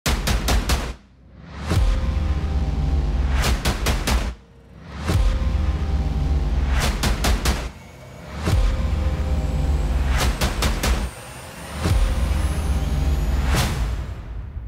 Get ready for the trailer of Kham Phaeng's elephantrescue journey is dropping tomorrow!
🎬 Tiny little sneak peek! Get ready for the trailer of Kham Phaeng's rescue journey is dropping tomorrow. 🐘✨ So don't forget to join us in reliving these amazing moments for Kham Phaeng's incredible journey!